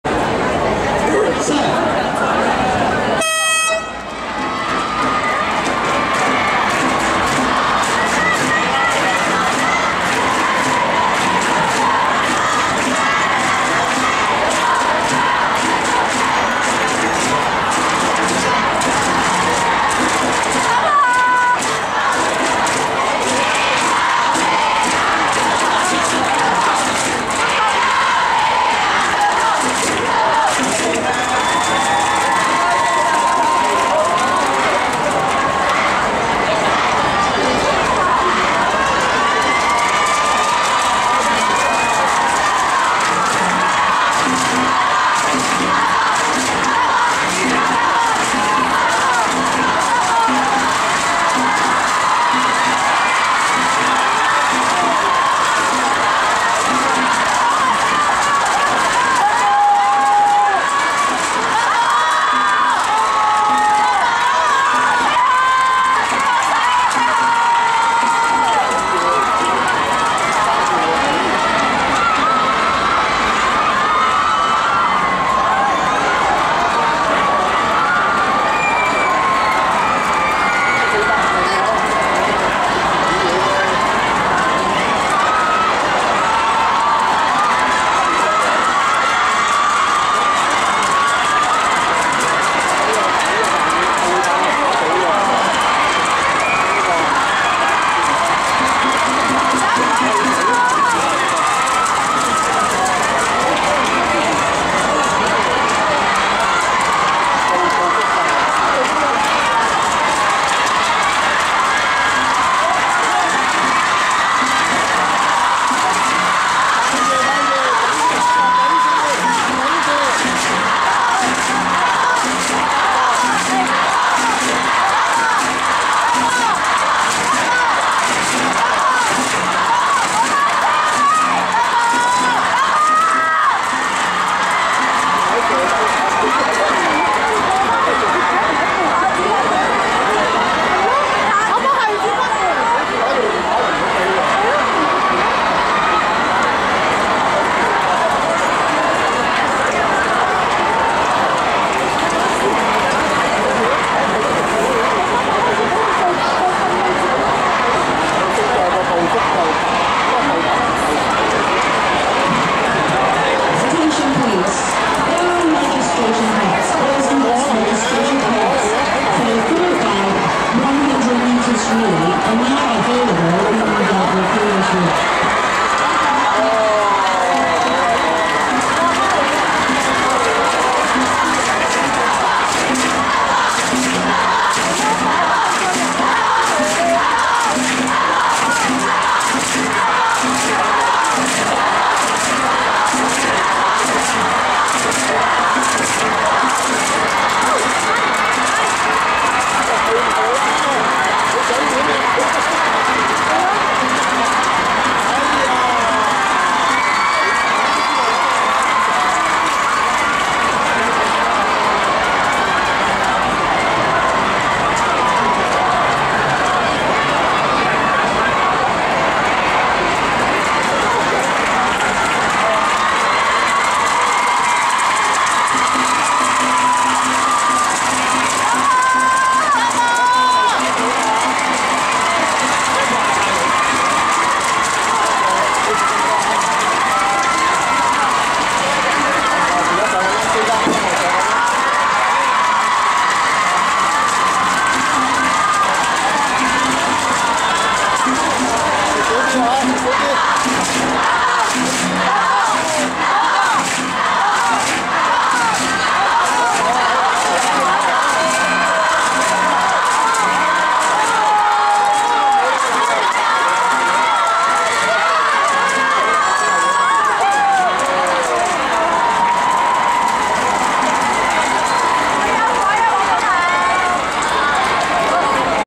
港九區D1中學學界田徑賽 2015-2016 GB 4X400m (Final)
1                           DGS       4:19.08
 2                           MCS      4:24.06
 3                           WIS       4:24.80
 4                           GHS      4:25.62
 5                           WFN      4:33.05
 6                           HYS       4:34.36
 7                           SPCS     4:37.66
 8                           SCGS     4:46.79